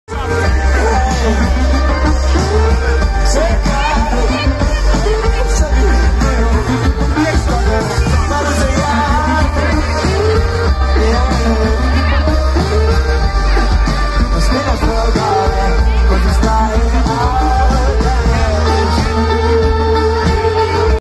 Opener Park Festival Gdynia Kolibki 23/07/2021 Dawid Podsiadło
koncert Dawida Podsiadło na Opener Park Festival Gdynia